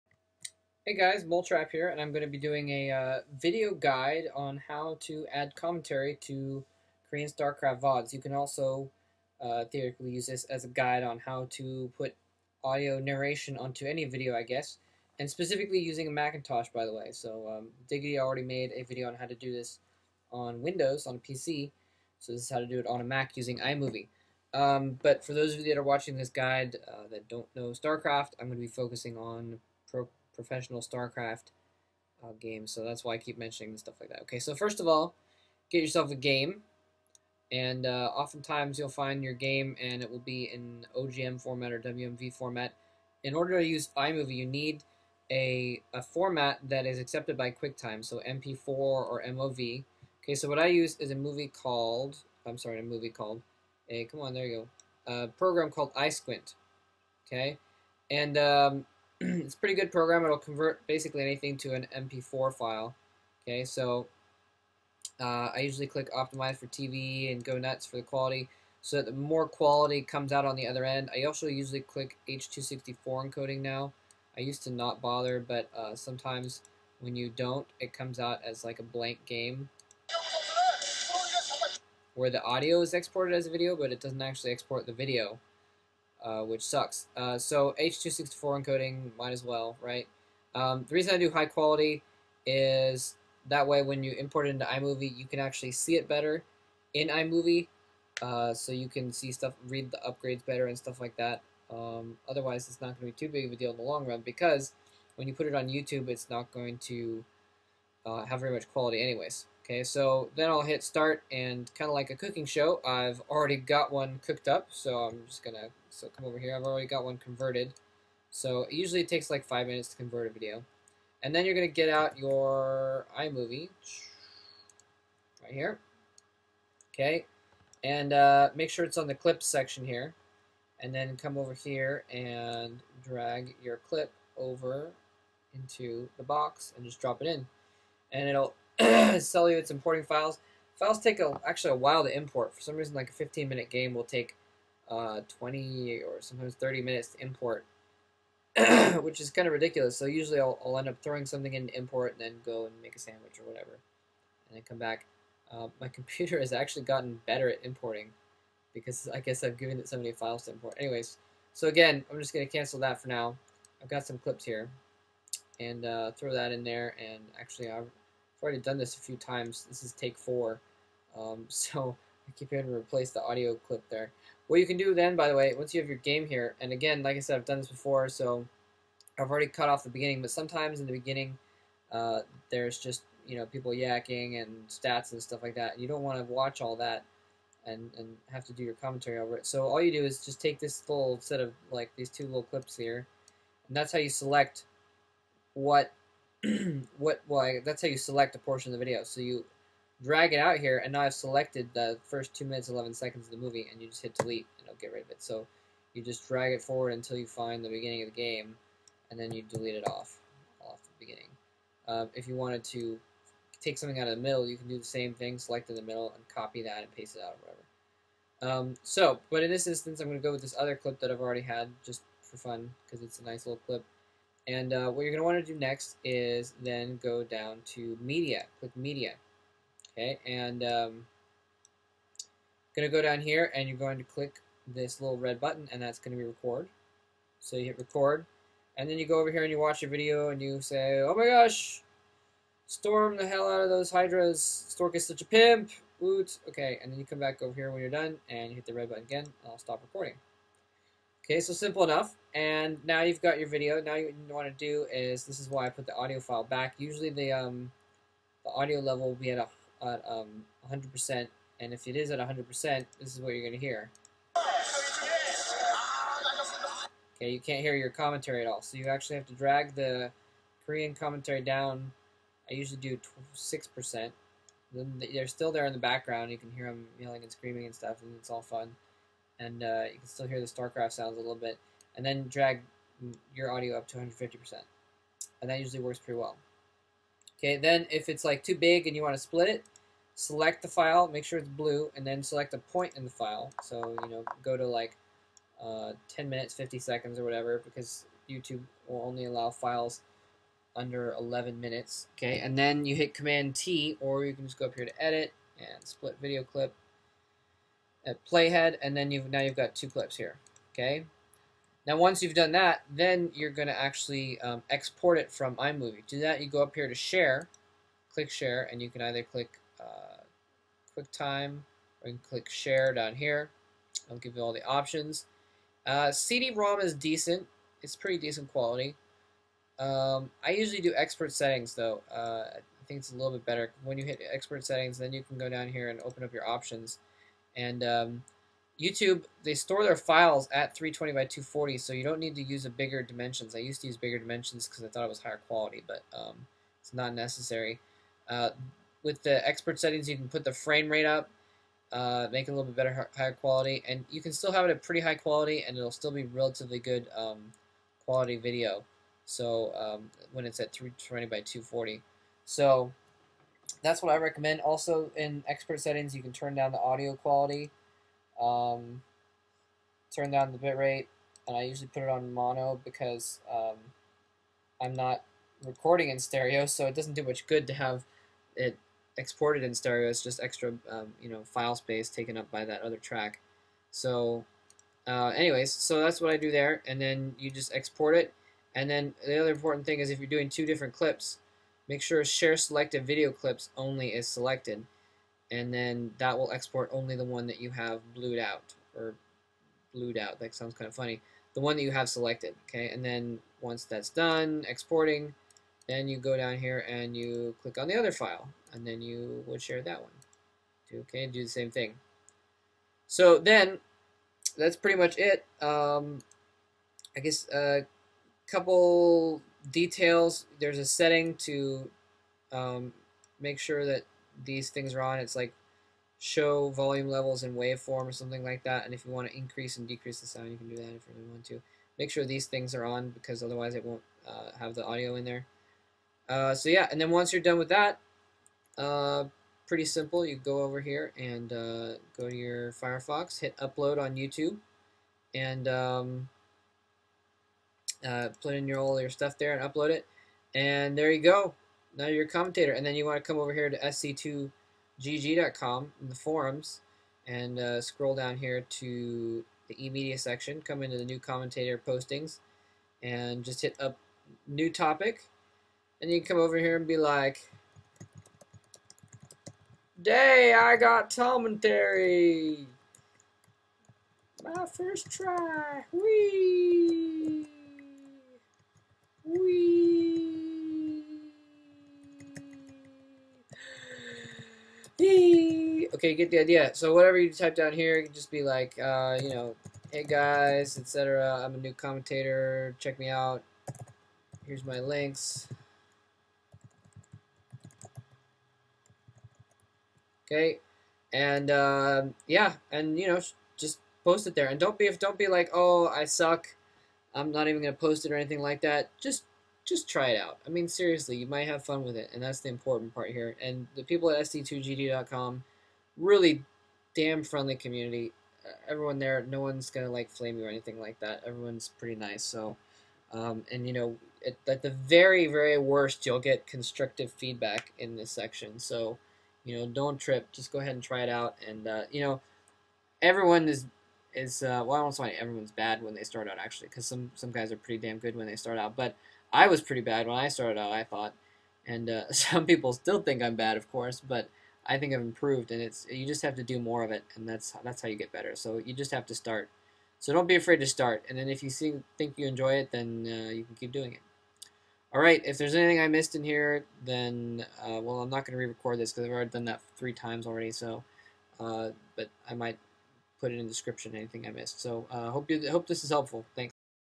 How to commentate a video on a Mac
A little video guide I made on how to make audio commentaries on Korean VODs on a Macintosh.

Special thanks to KeRo for showing me how to get everything set up!

If you know your pro Starcraft and think you might have fun, give commentary a try.

Let me know if there's any details I missed...